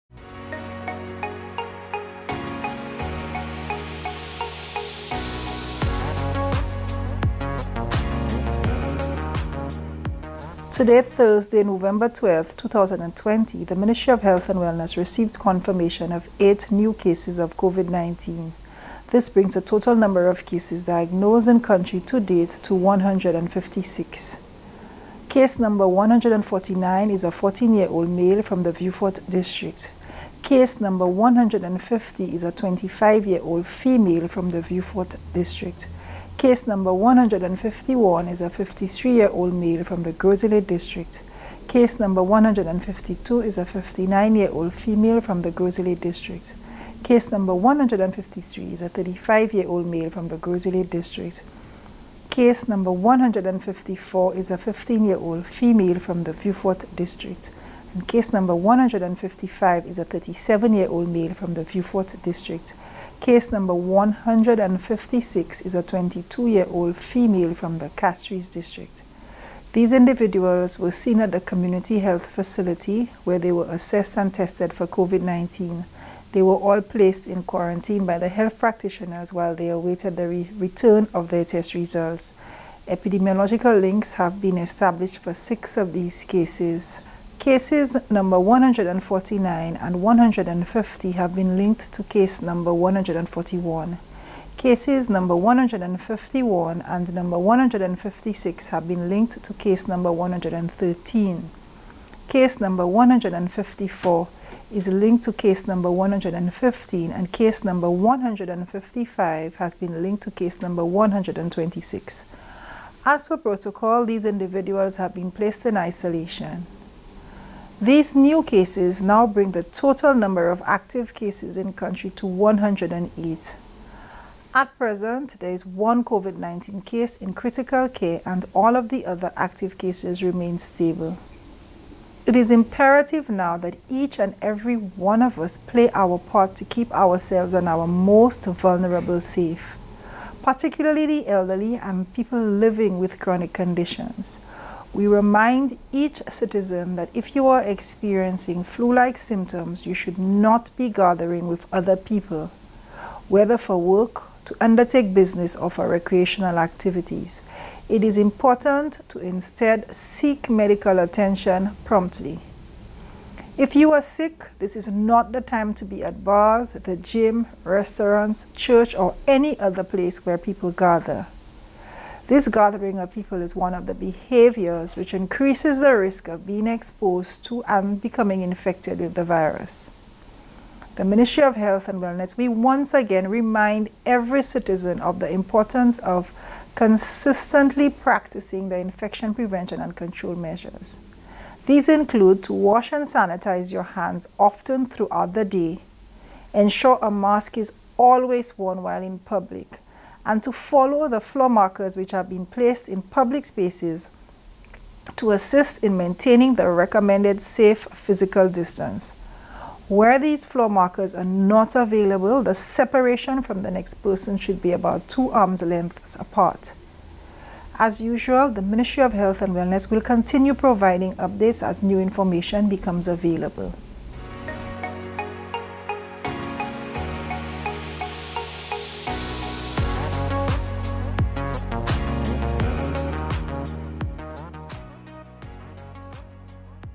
Saint Lucia records cases #149 to #156 of COVID-19
This brings the total number of cases diagnosed to 156; and the number of active cases to 108. It is imperative that each and every one of us play our part to keep ourselves and our most vulnerable safe, particularly the elderly and people living with chronic conditions. The gathering of people is one of the behaviours which increases the risk of exposure to and becoming infected with the virus.